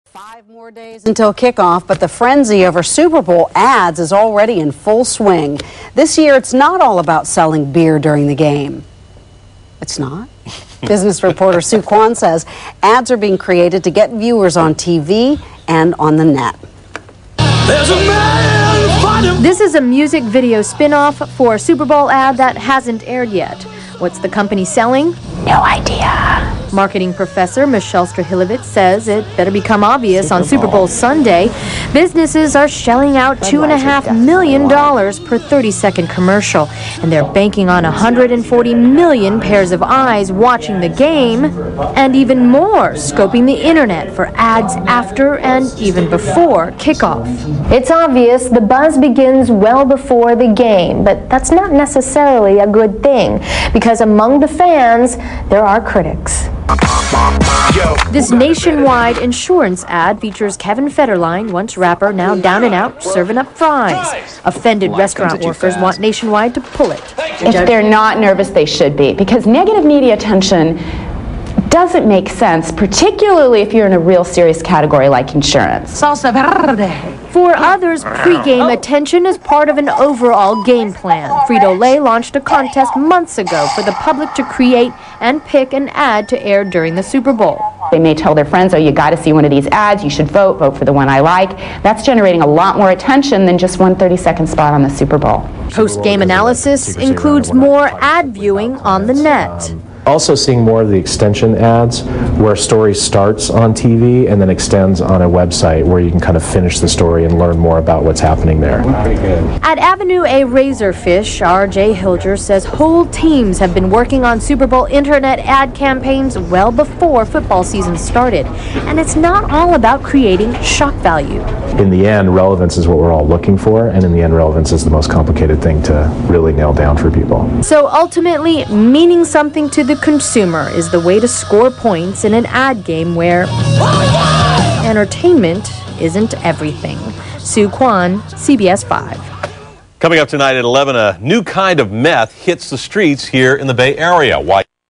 ABC7 News: 2007 Super Bowl Ad Frenzy- Michal Strahilevitz
Talking about the 2007 Super Bowl ads in advance on ABC News

Publication Date: January 31, 2007